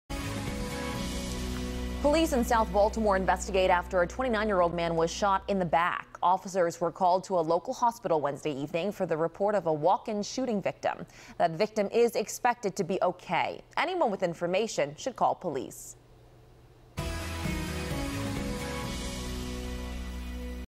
Man Shot In Back In South Baltimore
Police are investigating after a 29-year-old man was shot in the back in south Baltimore Wednesday.